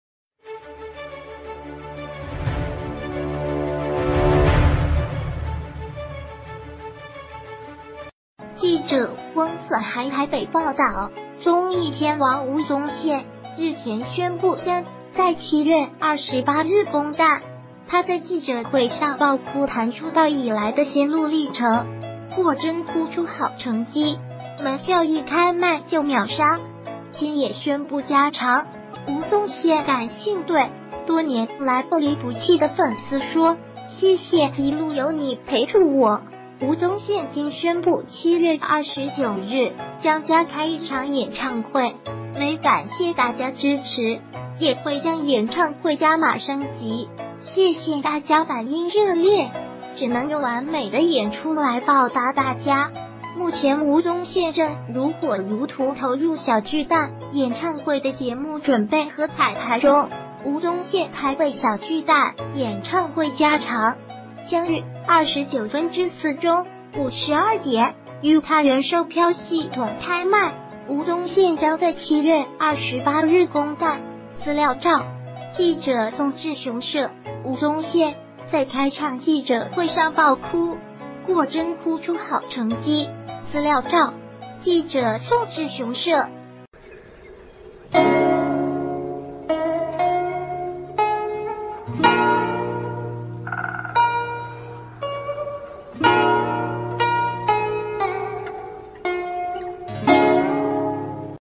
吳宗憲崩哭催票成功！爽爽宣布再加場
〔記者翁子涵／台北報...